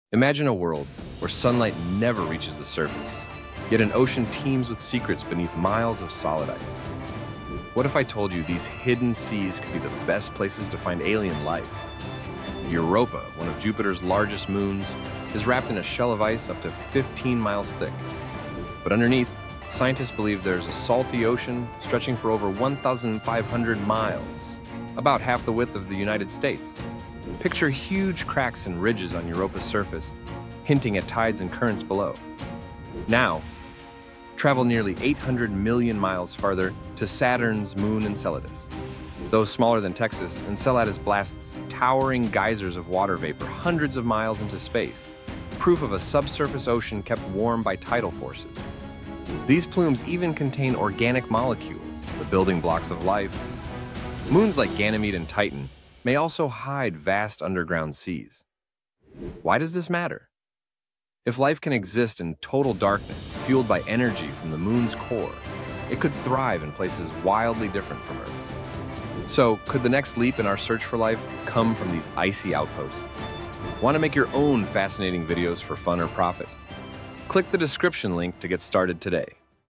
Hidden Oceans: Moons That Could Harbor Life
Beneath icy crusts, alien oceans stir. Could life thrive on Europa, Enceladus, or beyond? Dive into the secrets of our solar system!